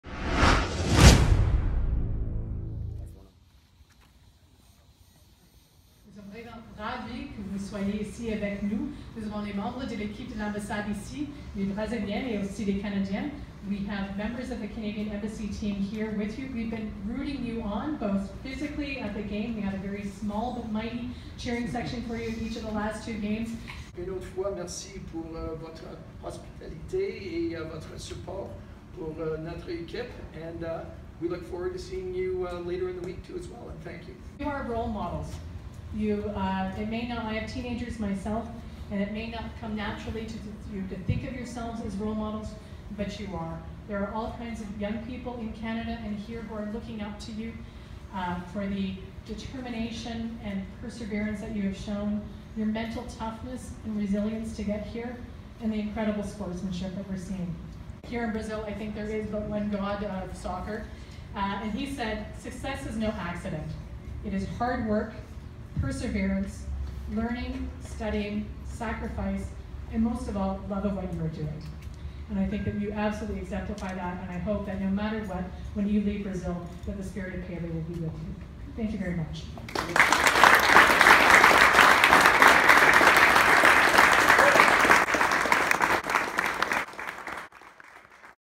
Canada U-17 visit to the Canadian Embassy in Brazil
'You are role models & Canadians are looking up to you.' Jennifer May greets canm17 at the Canadian embassy before their Friday group finale at the FIFA U-17 World Cup Brazil 2019